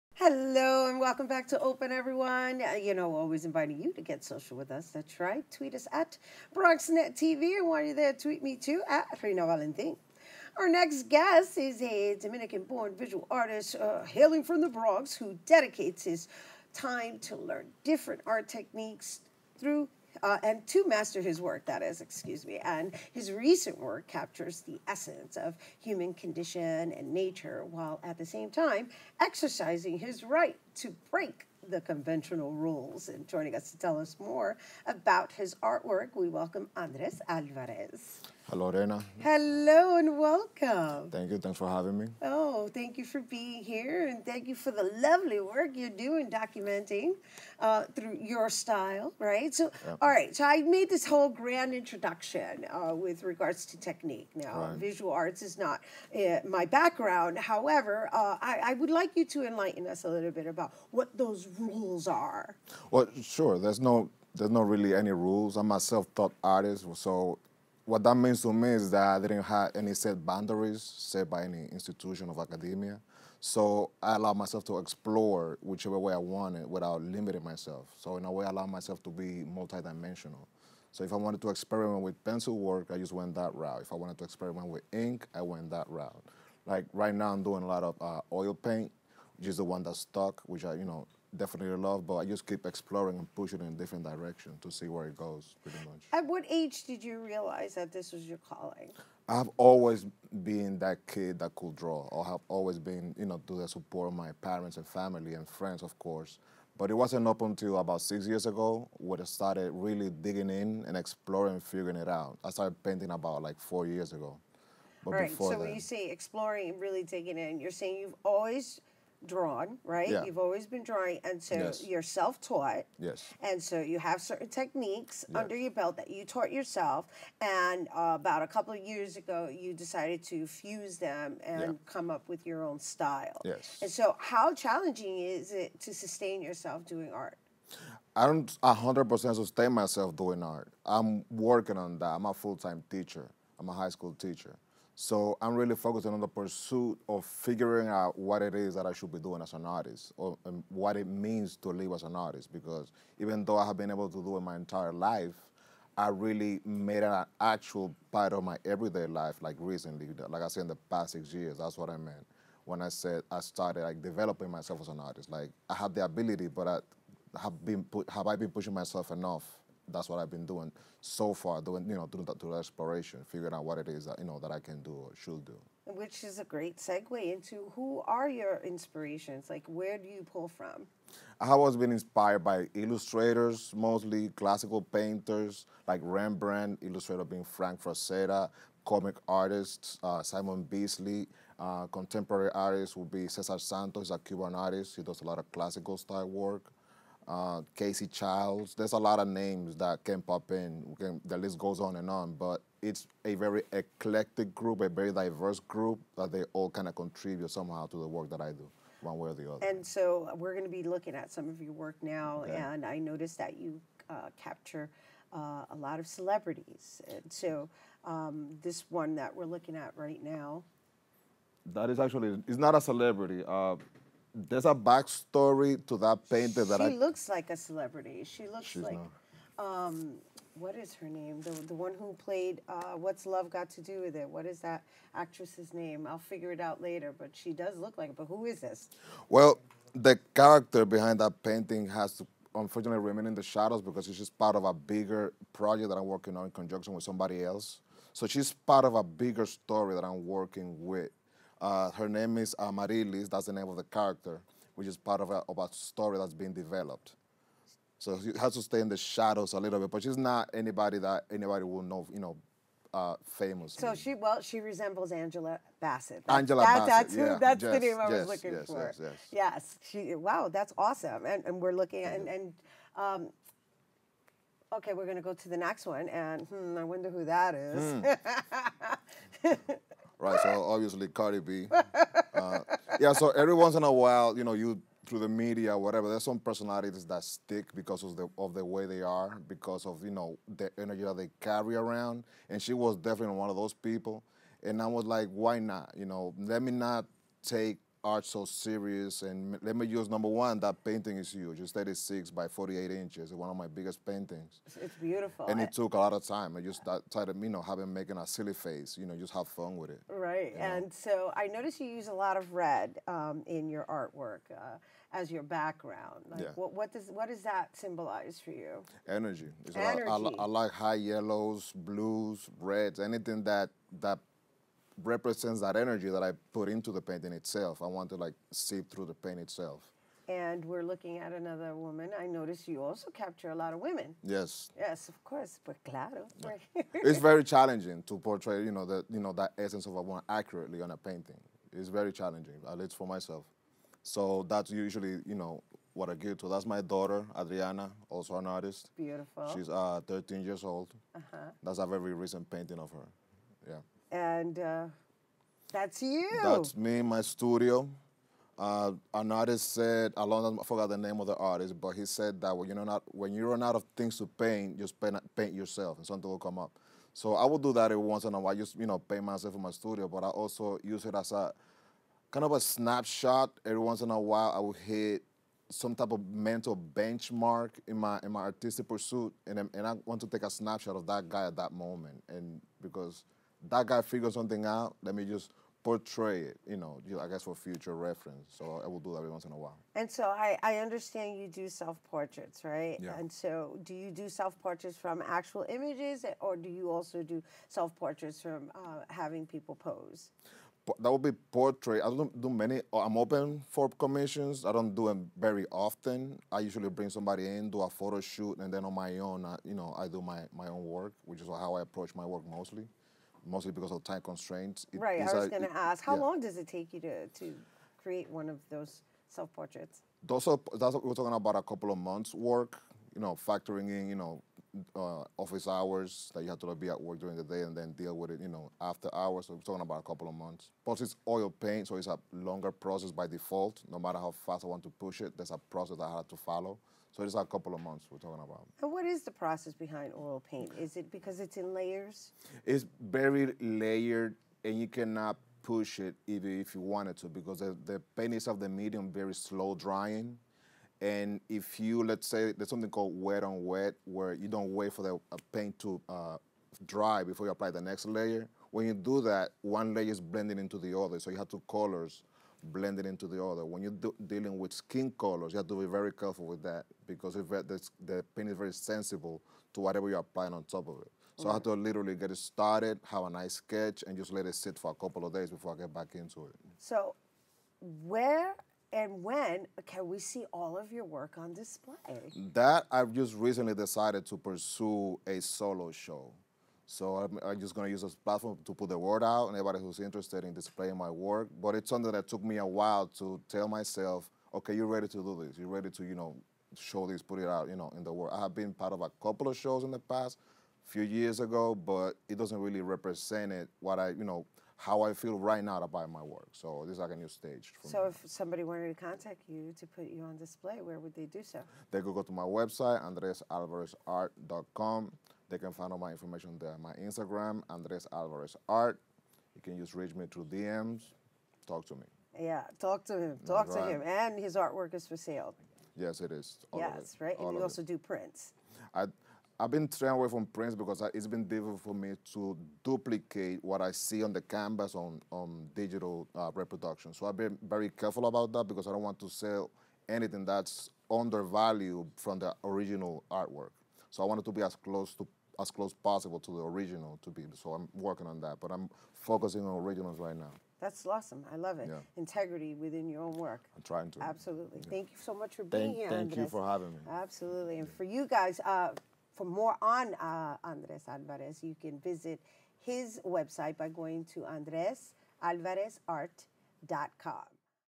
OPEN: Visual Artist Andres Alvarez
Host, Rhina Valentin sits down Visual Artist Andres Alvarez to discuss some of his original art pieces.